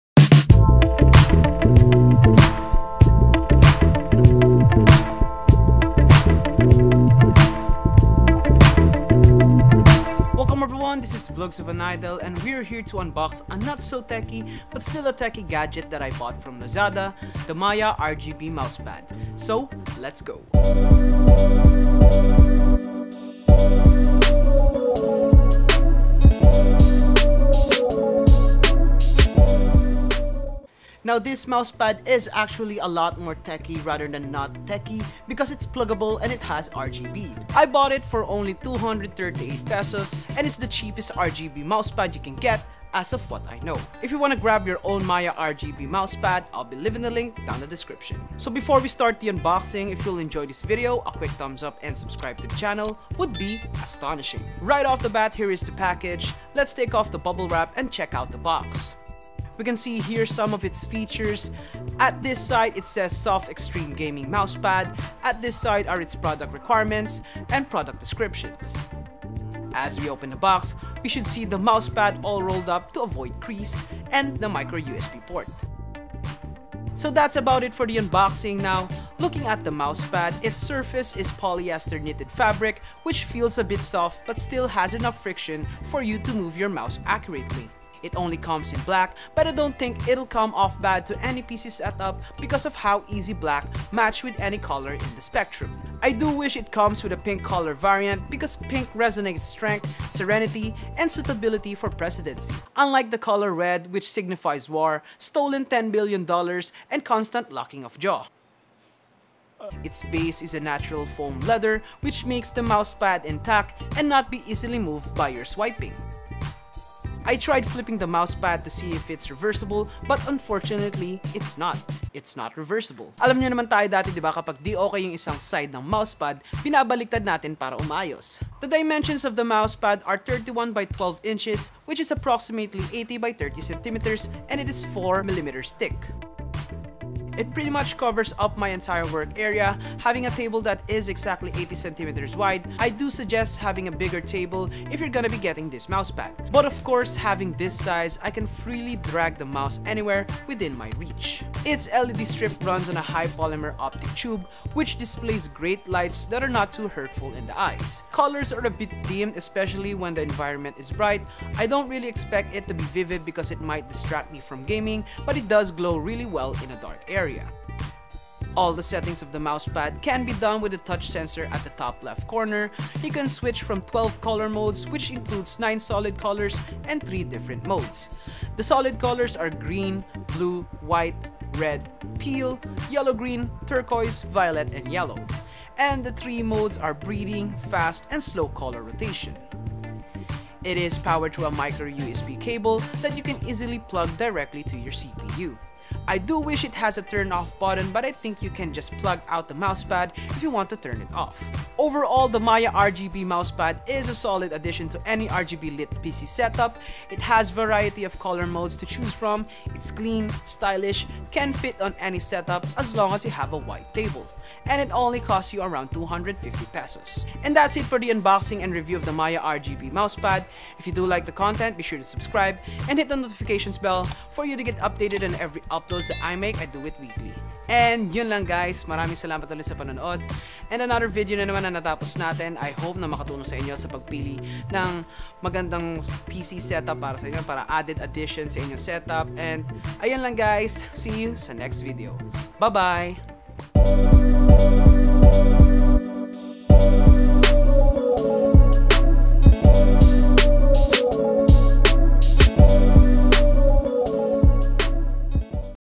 MAIA RGB Mouse Pad Unboxing and Review: Cheapest RGB Mouse Pad!
Welcome everyone we're back to unbox a not so techie but still a techie gadget, the MAIA RGB Mouse Pad. This might be the cheapest rgb mouse pad you can find, only costing around ₱238.

📸 on Canon EOS M50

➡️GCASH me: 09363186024

MY KIT?

🎙️Takstar SGC-600
📱Huawei Nova 5T
📸Xiaomi Yi Action Camera
🚫my balls